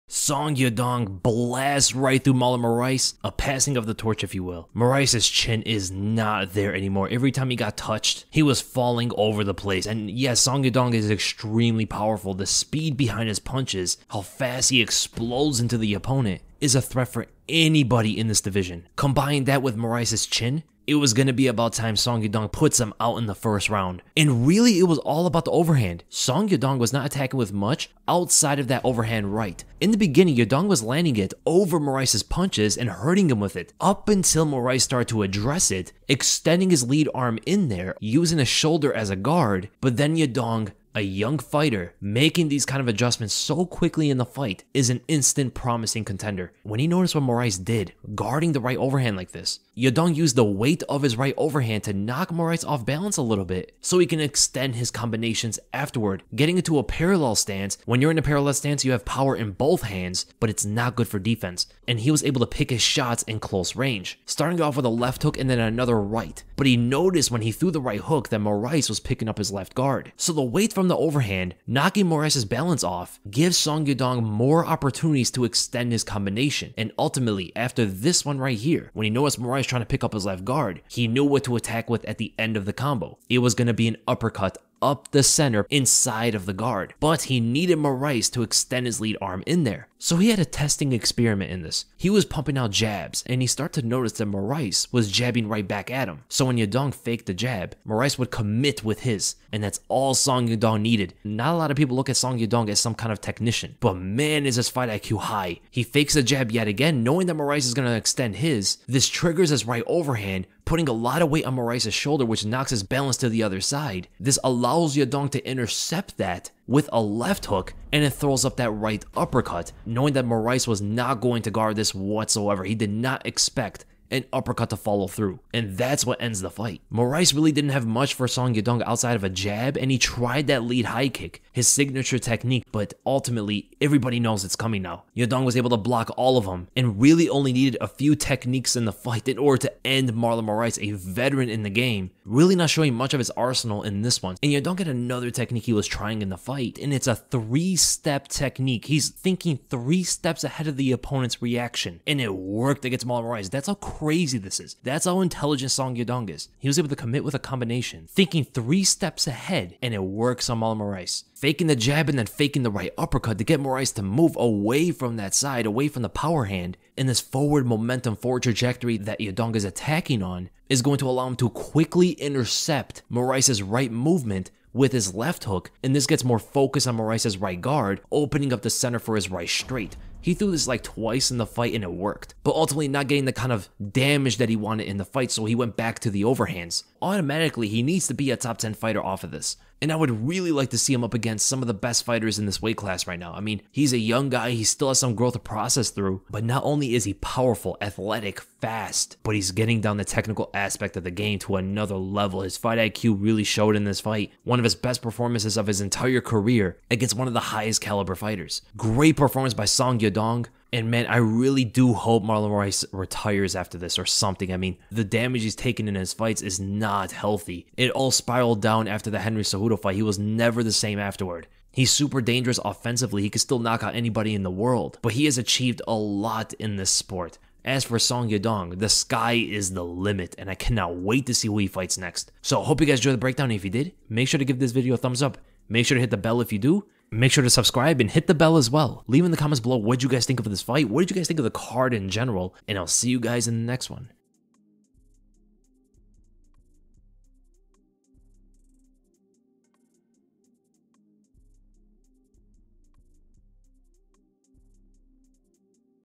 POWER!!! What Really Happened (Marlon Moraes vs Song Yadong)
Song Yadong vs Marlon Moraes was an explosive win for the prodigy. How did he set up the knockout against Moraes?